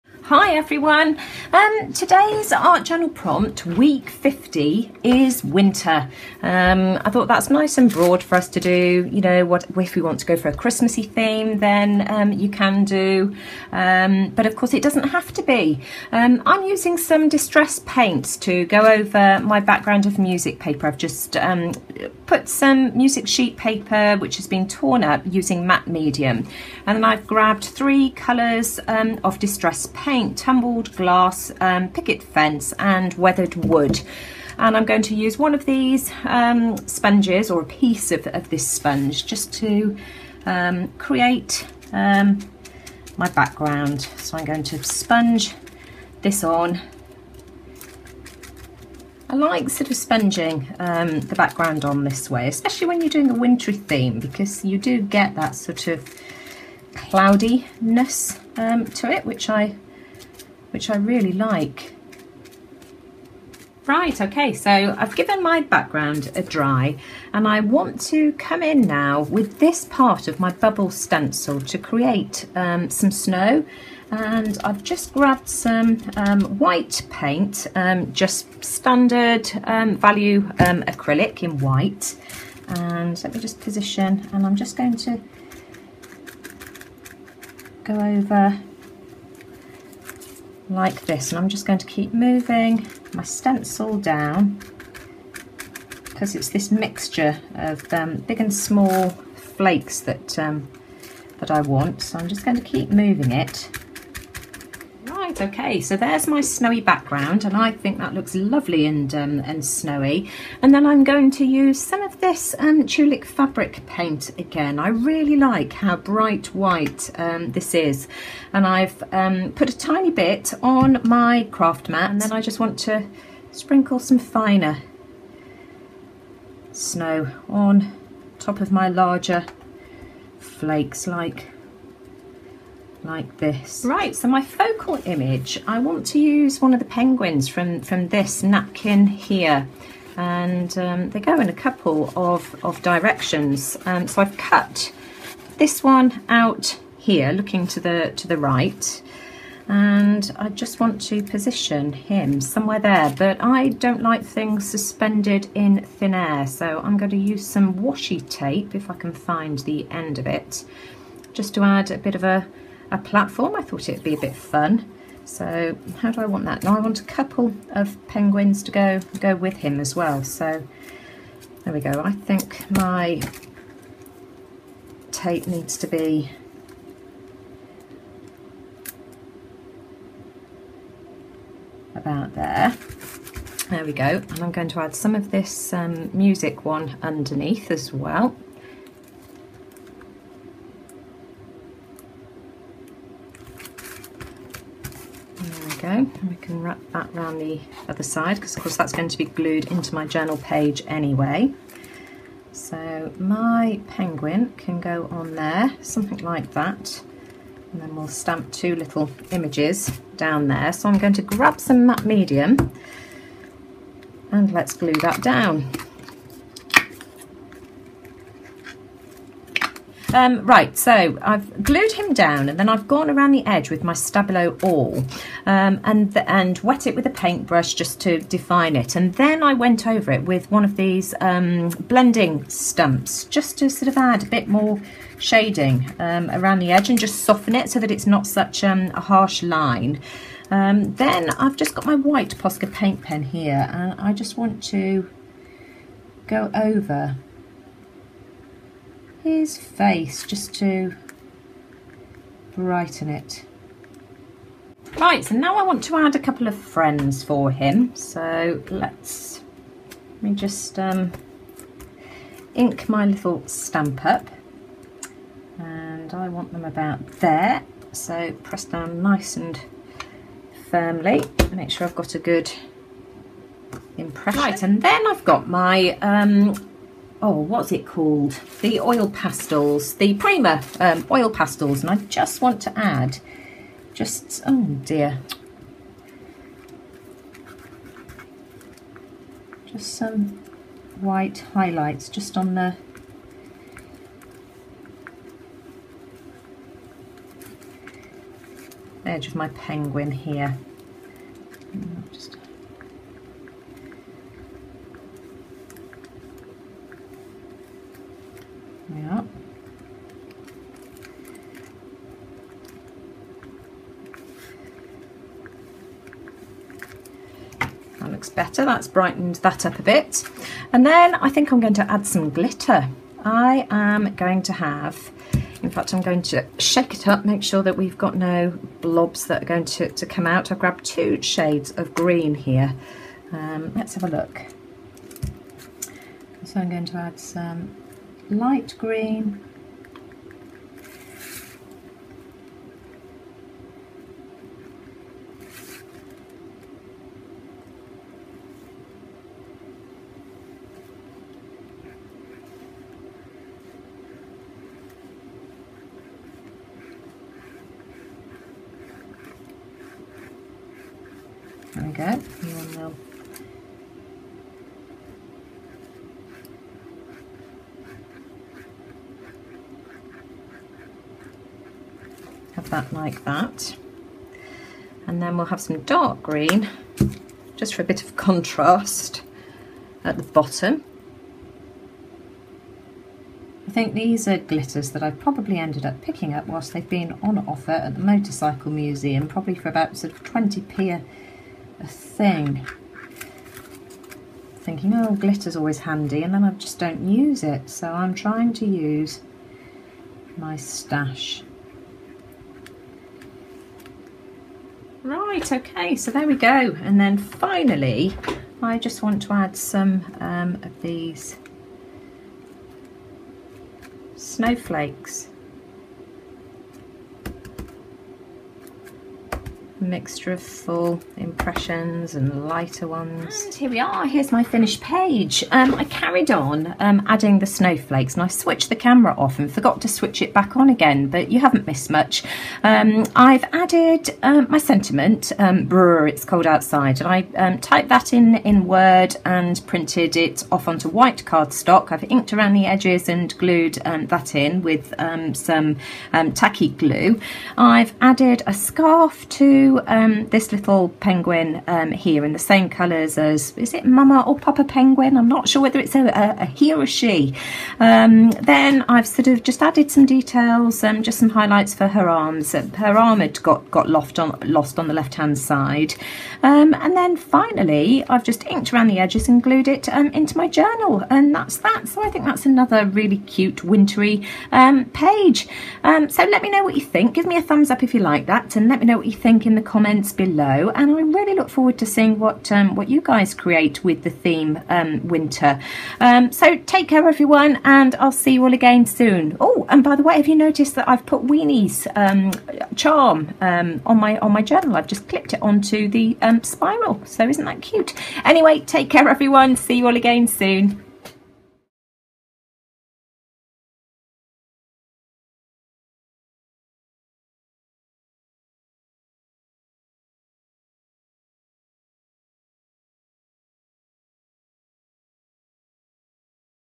Art Journal Prompts Week 50 - Winter
This weeks prompt is 'Winter'.  I have taken my inspiration from a napkin this week.  I look forward to seeing your wonderful artwork.